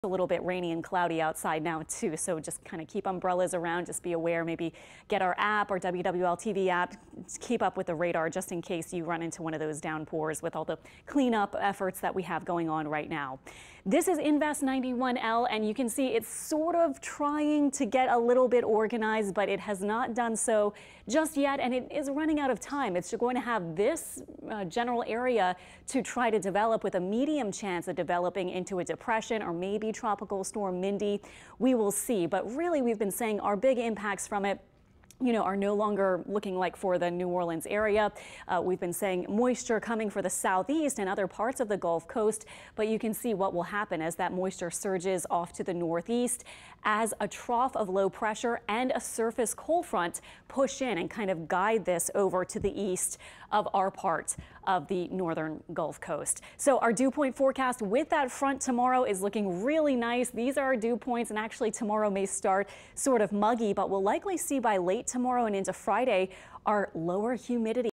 Wednesday midday tropical update: Invest 91 and Larry
Meteorologist Alexandra Cranford has an update on Wednesday, September 8, 2021.

Invest 91
The system south of the Louisiana coast is beginning to show signs of some limited organization and storms are developing around a general central location. It has a medium chance for development today as it moves towards Florida. Models are also a little more bullish with either a depression or storm forming as it heads toward the panhandle to big bend of Florida. It's also possible that it doesn't develop until once in the Atlantic. Whatever forms, it will be clear of Jacksonville by the time the Saints play Sunday.

Larry
A slowly weakening Larry could impact Bermuda with strong winds as it travels east of the island. Strong wind could also impact the Canadian Maritimes by Friday and the weekend as it transitions to an extra-tropical system.